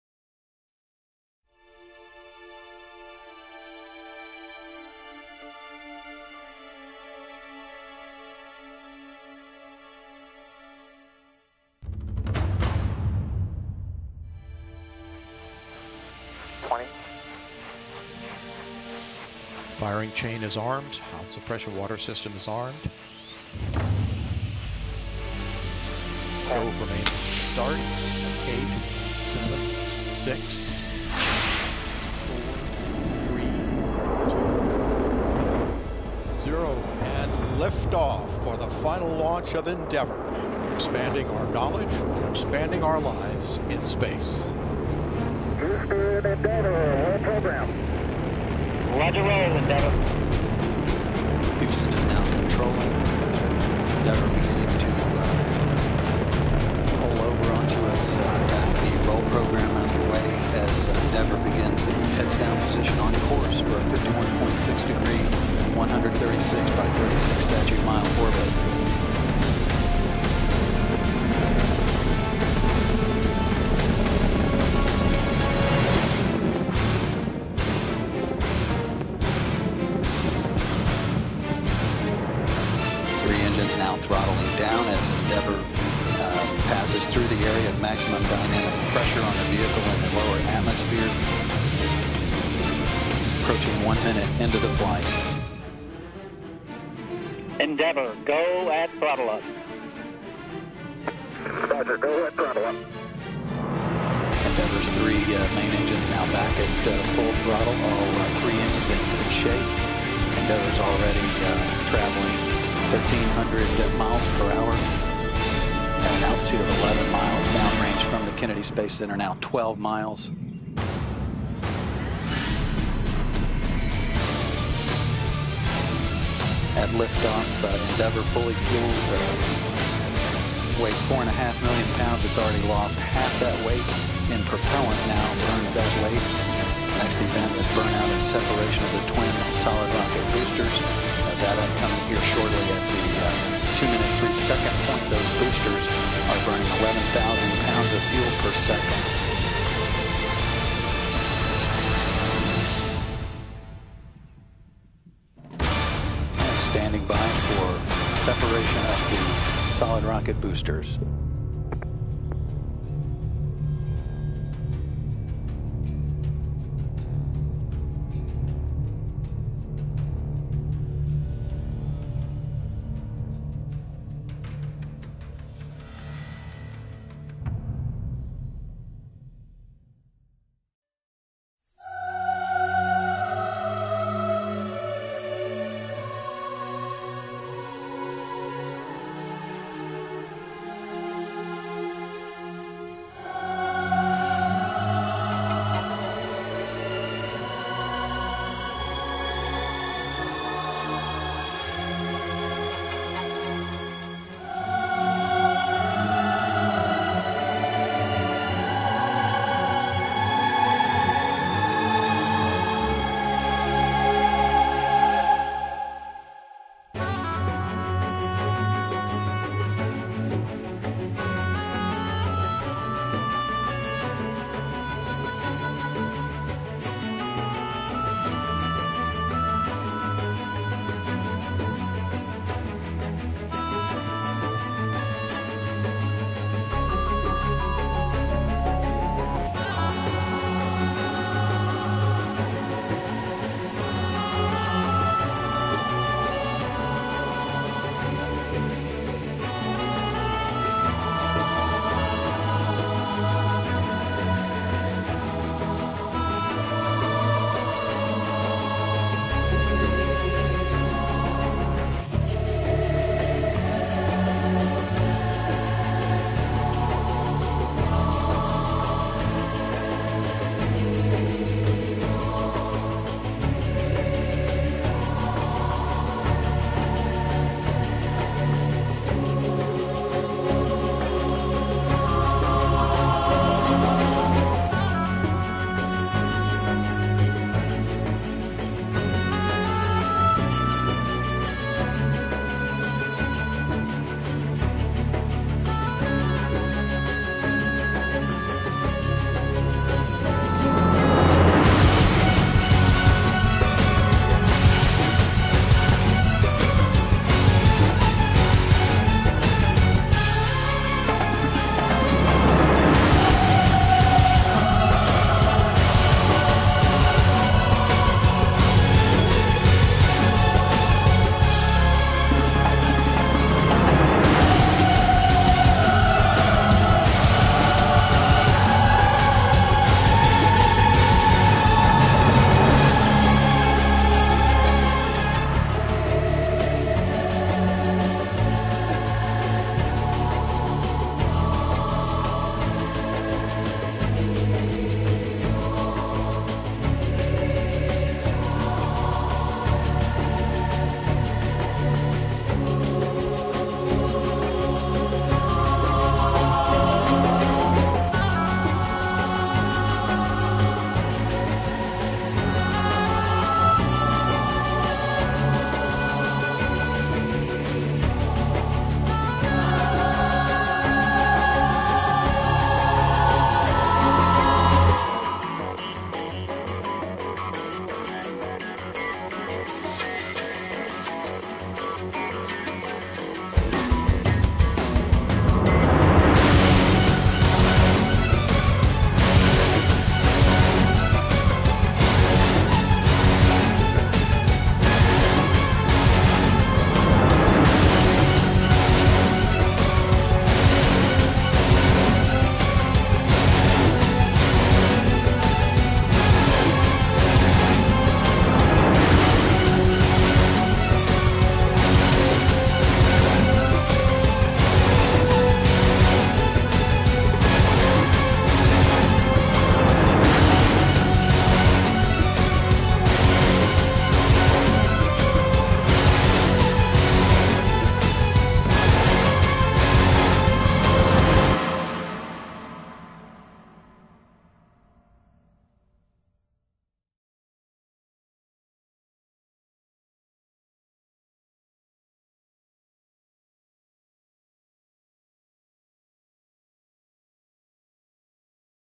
Views of Endeavour's Launch from Booster Cameras-1/6
Video recorded by cameras mounted on Endeavour's twin solid rocket boosters provide unique views of the May 16 launch.
STS-134 SRB Left Forward